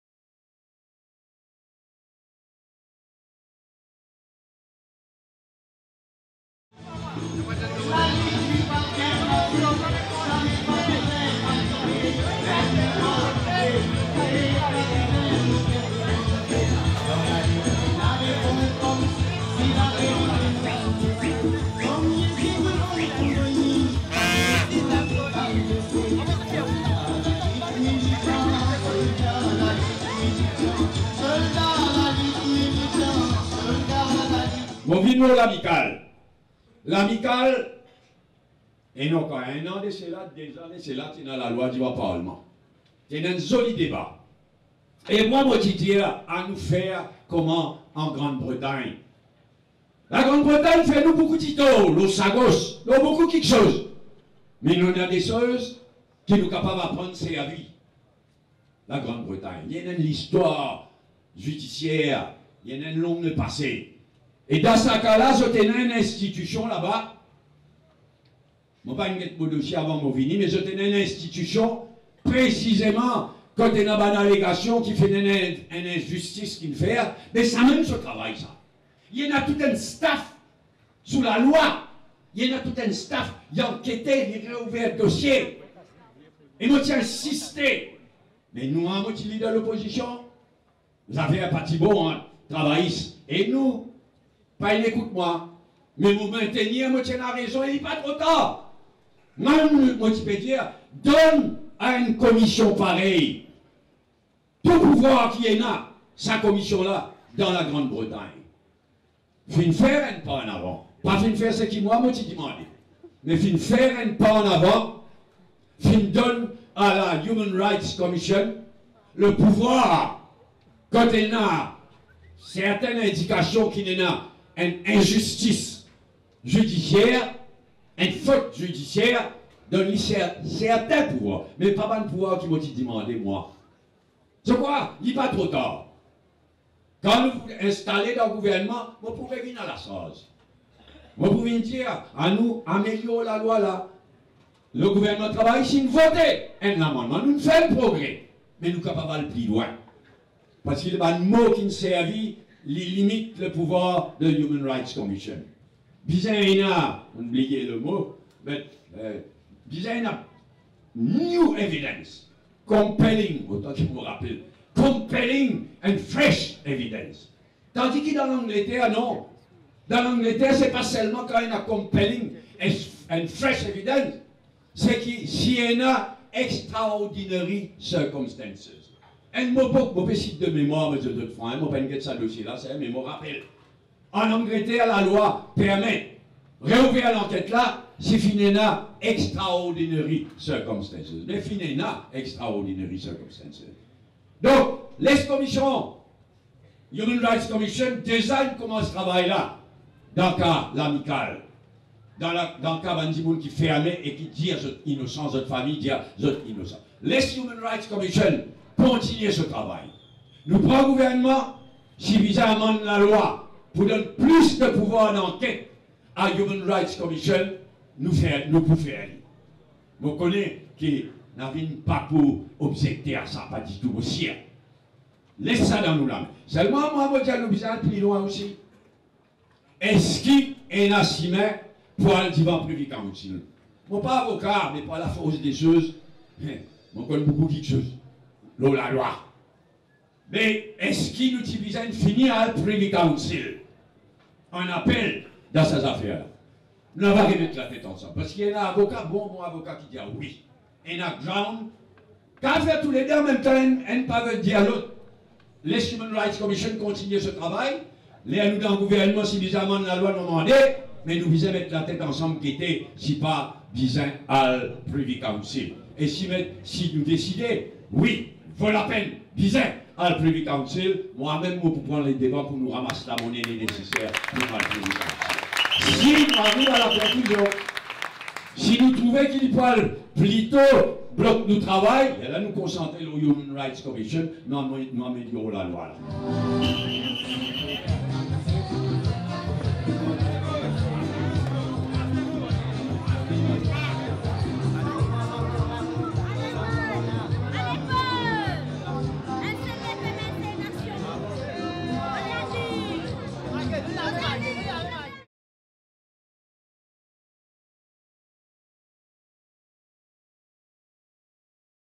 A Plaine-Verte: Bérenger promet de relancer le procès l’Amicale
Une réouverture du procès des quatre condamnés dans l’affaire l’Amicale sera pleinement appuyée par le gouvernement PTr-MMM a promis Paul Bérenger hier, mercredi 3 décembre, à Plaine Verte. Le leader mauve propose deux options pour rouvrir le dossier, soit le référer au Privy Council soit solliciter l’intervention de la Human Rights Commission dont les pouvoirs seront renforcés.